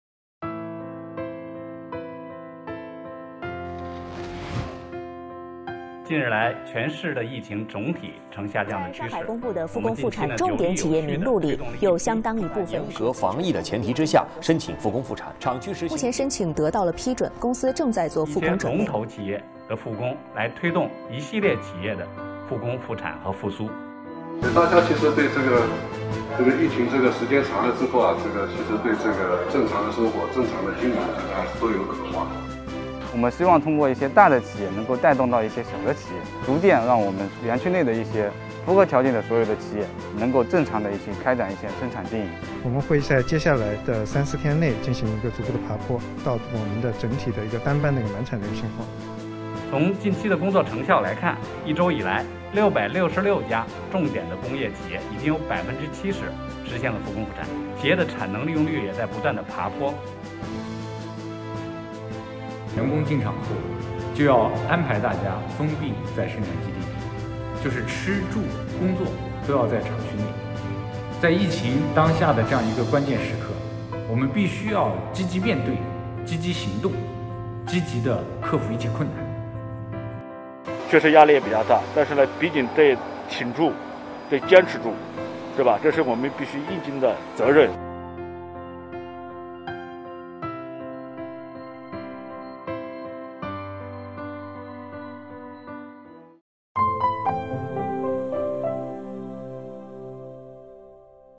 上海复工！
日前，上海市经信委发布《上海市工业企业复工复产疫情防控指引（第一版）》，上海分类分批推动具备封闭生产条件和疫情防控条件的重点企业复工复产。员工陆续到岗，机器高效运转……不少因疫情停产的工厂按下“重启键”。
        上海市副市长张为透露，近日来，上海全市疫情总体呈下降趋势，社会各界对复工复产的关注度越来越高。从近期工作成效来看，一周以来，666家重点企业已有70%实现了复工复产，企业的产能利用率也在不断提高。